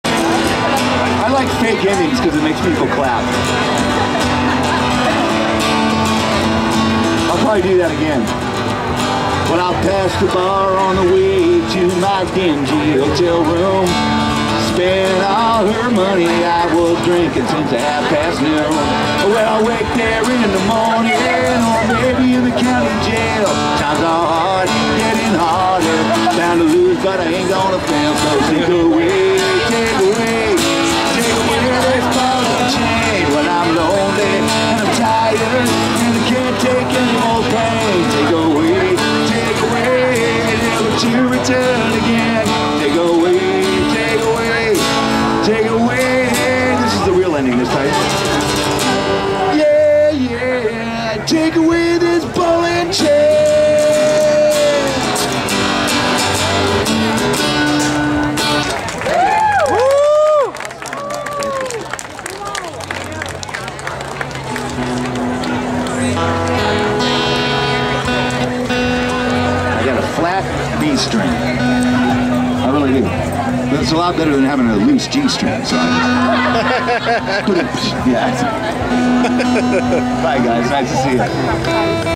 Man Playing his guitar with his feet 4.17.2011
Think You got it Rough..? Earthday Balboa Park San Diego.